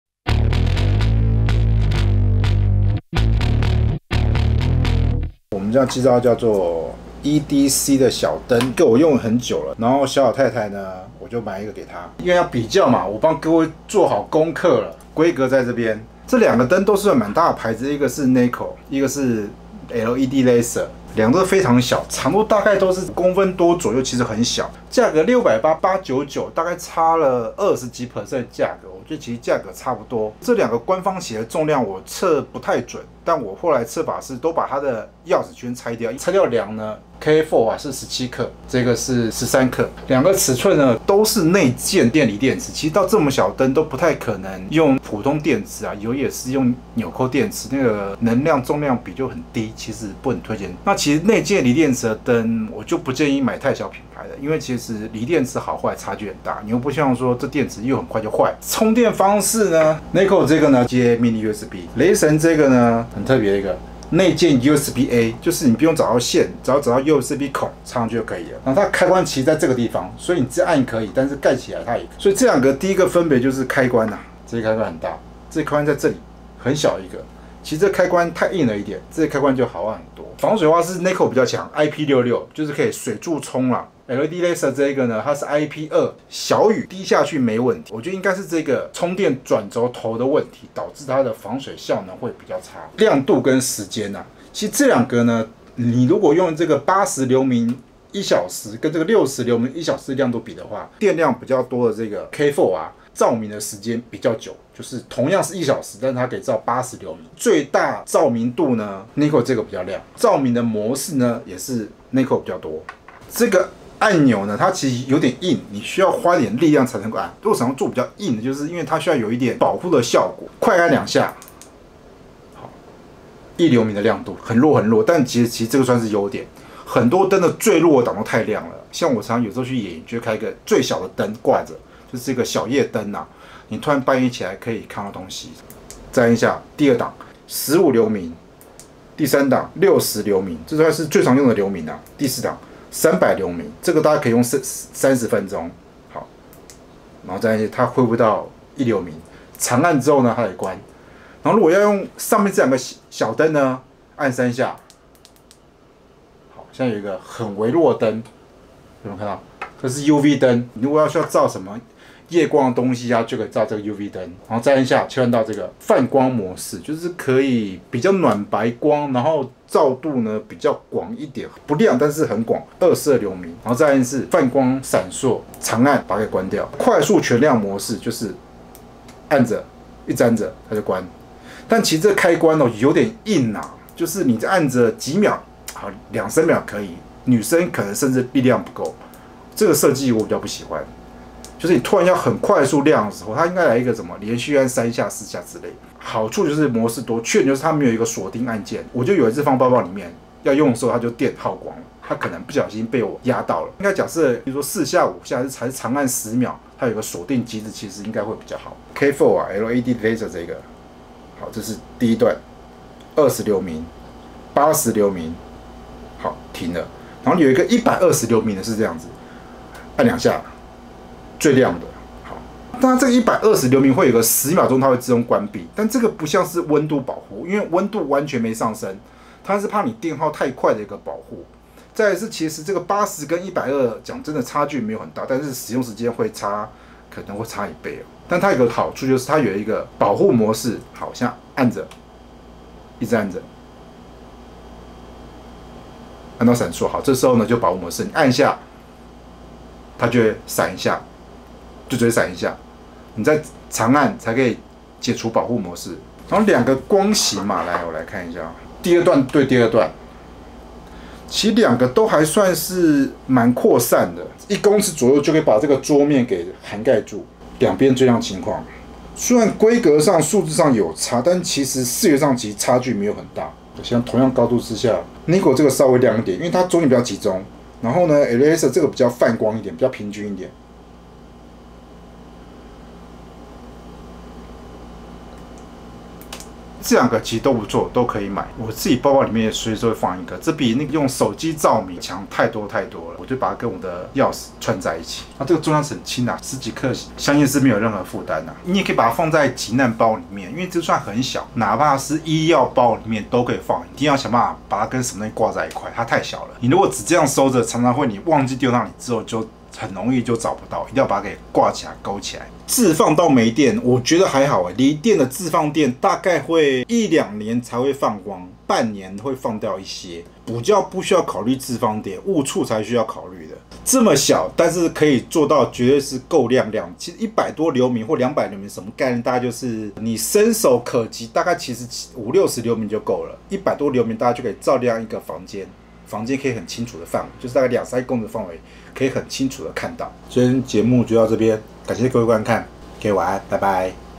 開箱比較超小型手電筒！充一次電就能用到4小時！Nitecore Tiki GITD ,Ledlenser K4R
我隨身都會攜帶一個小手電筒，晚上走夜路可以照明，包包找東西可以用，現在的鑰匙圈手電筒的亮度在鋰電池與高效率LED加持下，已經不輸一般的手電筒，當然高亮度使用時間還是會比較短，但是臨時使用一定夠用了，這次比較兩款超小型手電筒，分別是Nitecore Tiki GITD跟Ledlenser K4R，幫你比較優缺點，找出一款適合你的。

留言抽獎：只要留言的朋友就可以抽營火部落防火毯，雙平台各抽出一名。

除此之外都是盜版、竊取、仿冒，請留意以免受騙上當